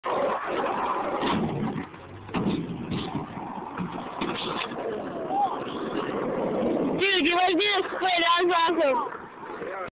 sizzam- Sam getting killed on rail
i jump up to do a sole grind and i missed and did the splits